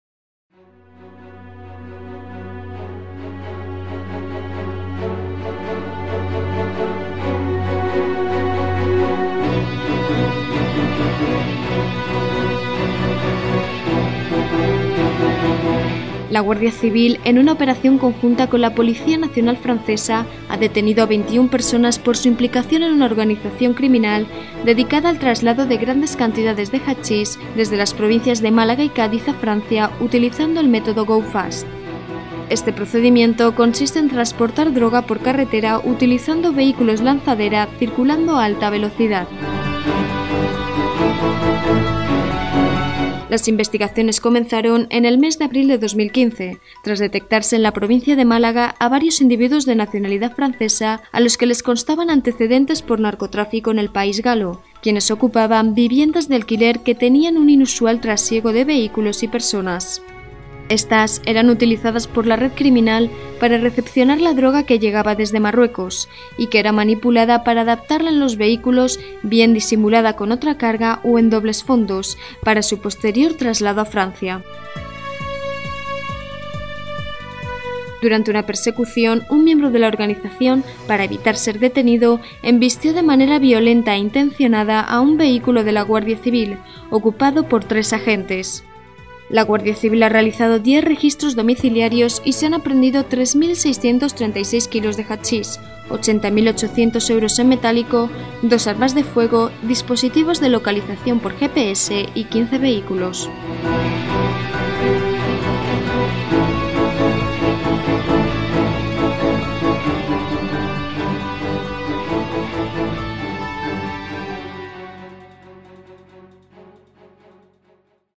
Operación Sartenazo. Operaciones de la Guardia Civil. Málaga
Operación conjunta de la Guardia Civil y de la Policía Nacional francesa

Desarticulada una red criminal dedicada al traslado de grandes cantidades de hachís a Francia utilizando el método “Go Fast”

Se ha detenido a  21 personas  y se han incautado más de 3,5  toneladas de hachís, 2 armas de fuego, 15 vehículos y más de  80.000 euros en efectivo, entre otros efectos

Uno de los detenidos, durante la detención, embistió con la furgoneta cargada de droga a una patrulla de la Guardia Civil, resultando heridos los ocupantes de ambos vehículos, que  tuvieron que ser hospitalizados

Madrid, 10 de marzo de 2016.-  La Guardia Civil, en una operación conjunta con la Policía Nacional francesa, ha detenido a 21 personas por su implicación en una organización criminal dedicada al traslado de grandes cantidades de hachís desde las provincias de Málaga y Cádiz a Francia utilizando el método “Go Fast” (transportan la droga por carretera utilizando vehículos lanzadera circulando a alta velocidad).

 Las investigaciones comenzaron en el mes de abril del pasado año, tras detectarse, en la provincia de Málaga, a varios individuos de nacionalidad francesa a los que les constaban antecedentes por narcotráfico en el país galo, quienes ocupaban una vivienda de alquiler que tenía un inusual trasiego de vehículos y personas.

 Posteriormente se localizaron otras tres viviendas, alquiladas por la organización con documentos de identidad falsos, cuya renta anual había sido abonada por anticipado con dinero en efectivo. Estas viviendas eran utilizadas indistintamente por la red criminal para recepcionar la droga que llegaba desde Marruecos y que era manipulada para adaptarla en los vehículos, bien disimulada con otra carga o en dobles fondos, para su posterior traslado a Francia por el método conocido como  “Go-Fast”.

 Durante el mes de agosto fue interceptado en Francia uno de sus vehículos con 230 kilos de hachís y, tras tres registros, se procedió a la detención de cinco personas relacionadas con el referido transporte.

  En  el mes de octubre se interceptaron dos furgonetas de la organización que habían salido de una de las viviendas localizadas y que, supuestamente, iban cargadas de droga con destino Francia. Ambas furgonetas iban precedidas por otro turismo lanzadera, cuyo objetivo era dar aviso de posibles controles policiales.

 Interceptación de los vehículos

 Debido a la peligrosidad con la que operaba la red se intentó dar el alto a los vehículos aprovechando la parada en una cabina del peaje de una autopista, pero éstos, al detectar la presencia policial, se saltaron las barreras de seguridad e iniciaron la huída a gran velocidad. Decenas de kilómetros más tarde, en la localidad malagueña de Villanueva del Trabuco, el conductor de una de las furgonetas, para evitar ser detenido, embistió de manera violenta e intencionada a un vehículo de la Guardia Civil ocupado por tres agentes. Los guardias civiles y el conductor de la furgoneta tuvieron que ser hospitalizados por las heridas que presentaban.

 Finalmente todos los vehículos fueron interceptados y sus conductores detenidos, incautándose entre las dos furgonetas 67 fardos de hachís, que arrojaron un peso aproximado de 2.080 kilos.

 Además se realizaron varios registros domiciliarios en distintas localidades de Málaga y Cádiz, en los que se intervinieron 1.258 kilos más de hachís en dos de las viviendas que hacían funciones de “guardería” (vivienda donde se oculta la droga hasta ser transportada).

 Red criminal perfectamente organizada

 Los cabecillas eran los encargados de crear y mantener la infraestructura necesaria para la adquisición de droga, el traslado a las “guarderías”, su manipulación y el posterior envío a Francia, donde la  comercializaban y distribuían. Este extremo ha sido confirmado por la Policía Nacional francesa, que desplazó varios agentes hasta Málaga para colaborar estrechamente en esta operación.

 La operación se ha desarrollado por el Equipo de Delincuencia Organizada (EDOA) de la Unidad Orgánica de Policía Judicial (UOPJ) de la Comandancia de la Guardia Civil de Málaga en las provincias de Cádiz, Málaga y varias localidades francesas donde se han realizado 10 registros domiciliarios y se han aprehendido 3.636 kilos de hachís, 80.800 euros en metálico, 2 armas de fuego, dispositivos de localización por GPS y 15 vehículos, entre otros efectos.